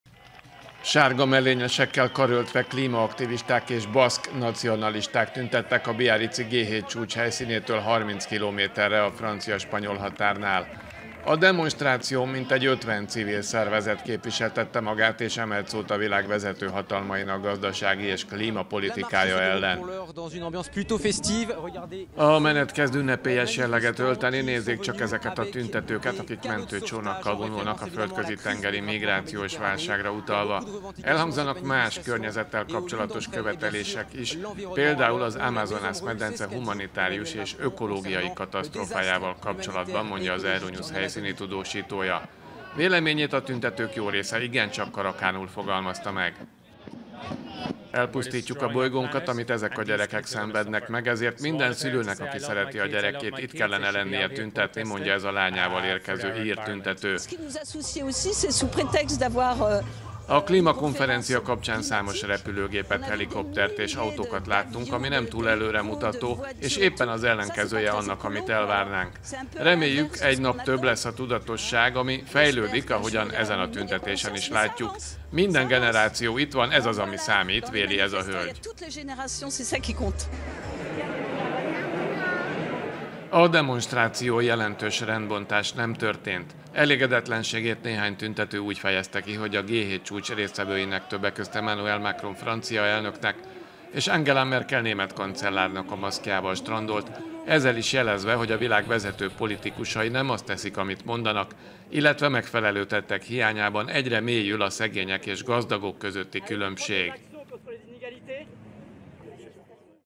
G7: Tüntetés a spanyol-francia határon
A tüntetők elégedetlenségüket fejezték ki a világ vezető politikusainak klímapolitikájával szemben.…